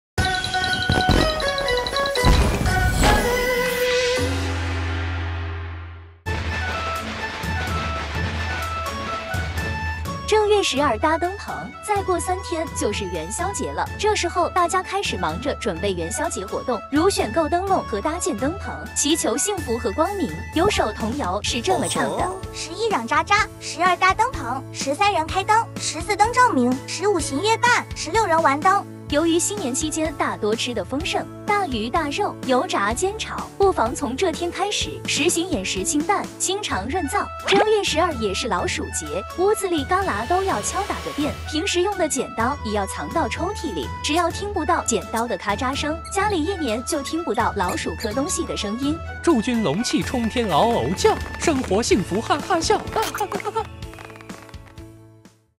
旺旺一条龙 | 年十二 准备元宵搭灯棚
开始倒数元宵节！大鱼大肉好几天，今天不妨吃得清淡些。